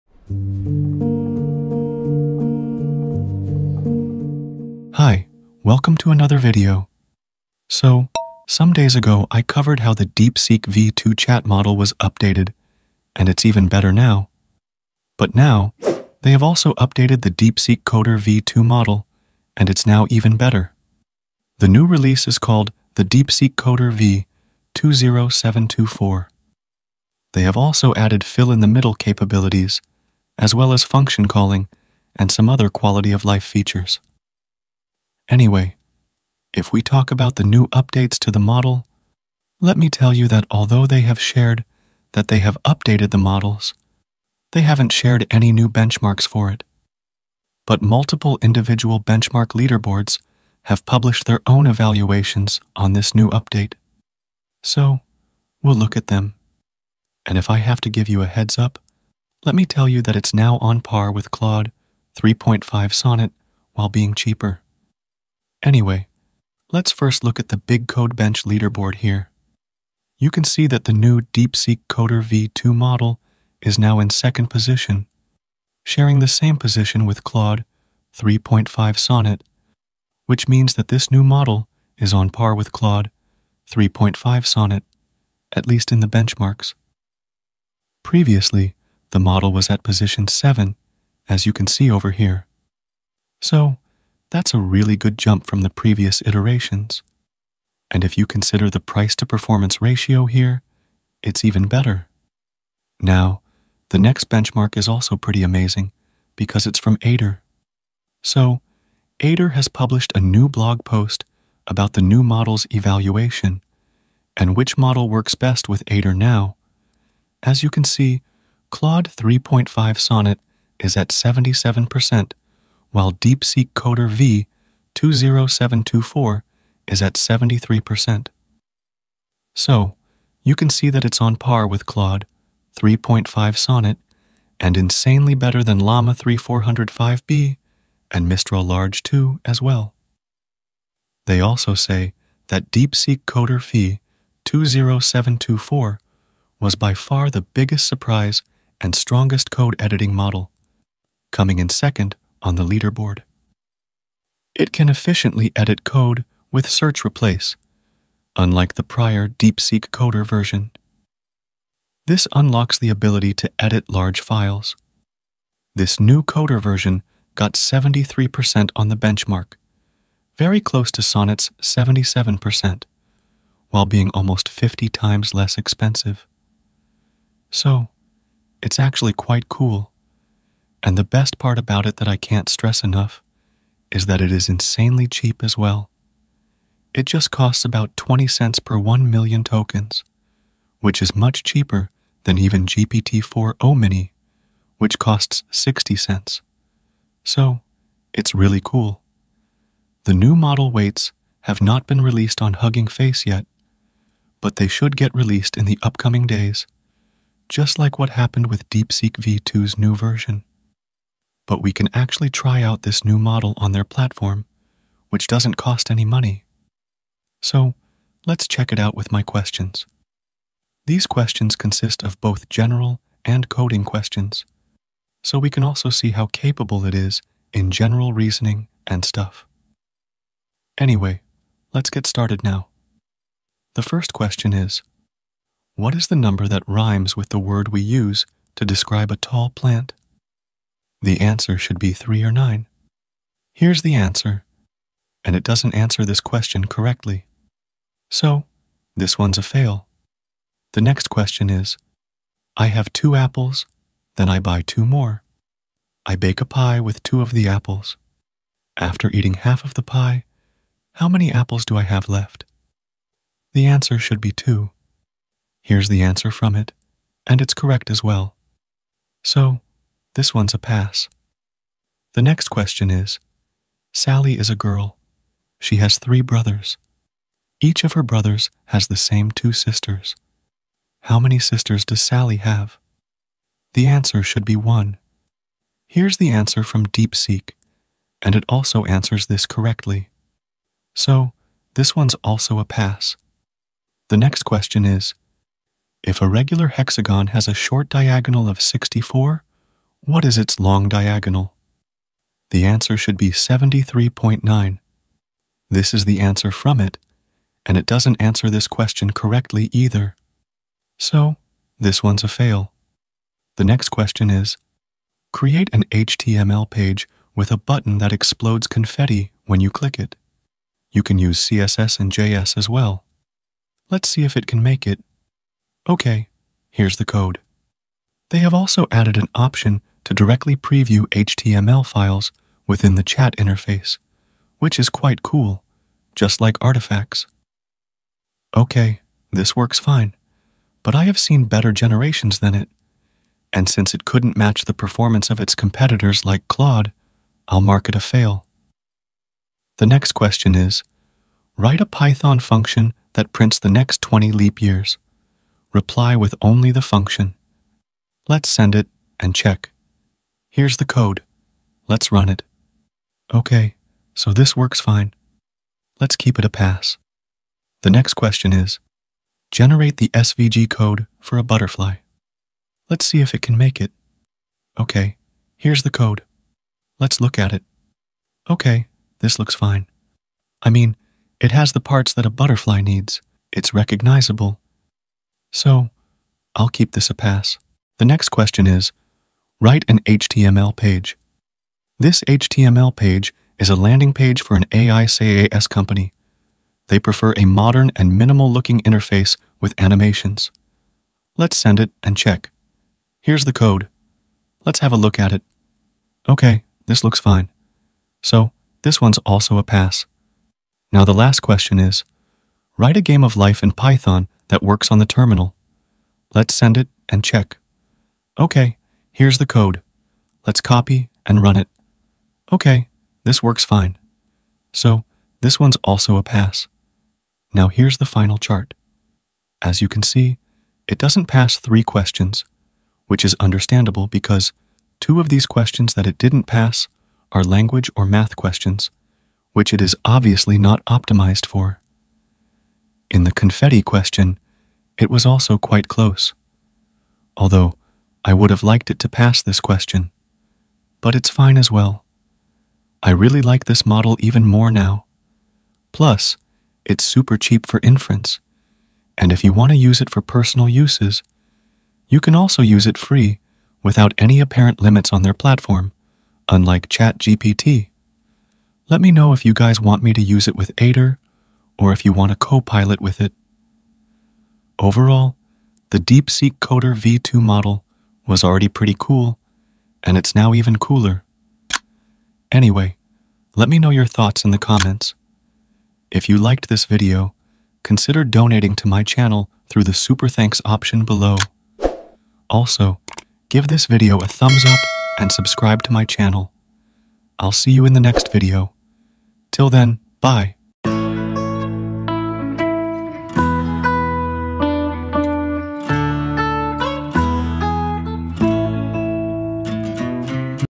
DeepSeek-Coder-V2.1 (0724) : BEST CODING LLM JUST GOT BETTER! (Fully Tested & Beats Claude, GPT-4O)
In this video, I'll be telling you about the new updates to DeepSeek-Coder-V2 model. This new update is numbered 0724. The new updates make the DeepSeek-Coder-V2 model insanely better at coding skills & other skills. This is a Mixture-of-Experts model with 128K Context Limit and it is really good. In my testing, it beats Llama-3.1 405B, GPT-4O, Claude 3.5 Sonnet, Qwen-2, DeepSeek-V2, Llama-3 & Others. It is even better in Coding Tasks and is also really good at doing Text-To-Application, Text-To-Frontend and other things as well. I'll be testing it to find out if it can really beat other LLMs and i'll also be telling you that how you can use it. This model is also insanely cheaper than every other model including GPT-4O Mini and can also be used for free.

Key Takeaways:

📈 DeepSeek-Coder V2 is Here! The latest version, DeepSeek-Coder-V2-0724, is now available with enhanced capabilities, outperforming previous iterations and giving strong competition to Claude 3.5 Sonnet. 🚀

🔍 New Features Unveiled: The update includes fill-in-the-middle capabilities and function calling, offering significant quality-of-life improvements for developers and AI enthusiasts. 🛠️

📊 Benchmark Triumphs: Multiple benchmark evaluations show DeepSeek-Coder V2 performing on par with leading models like Claude 3.5 Sonnet, making it a formidable competitor in the AI coding space. 🥈

💸 Affordable Excellence: This model offers a remarkable price-to-performance ratio, costing only 20 cents per million tokens, making it much cheaper than GPT-4O-mini. 💵

👩‍💻 Versatile Performance: Whether it's coding questions or general reasoning, DeepSeek-Coder V2 delivers reliable results, demonstrating its broad capabilities and efficiency. 🧠

✨ Aider Endorsement: Aider's latest blog highlights DeepSeek-Coder V2 as a top-performing model, especially for code editing, and praises its cost-effectiveness. 🌟

💻 Try it for Free: You can test out DeepSeek-Coder V2 on their platform without any cost, allowing you to experience its features and capabilities firsthand. 💯

Timestamps:

00:00 - Introduction
00:08 - About New Updates to DeepSeek-Coder-V2 (0724)
01:06 - Benchmarks & Leaderboard (Beats Claude-3.5 Sonnet & Llama-3.1 405B)
01:47 - Aider Benchmarks (Beats Claude-3.5 Sonnet & Llama-3.1 405B)
03:40 - Testing
07:07 - Conclusion
08:01 - Ending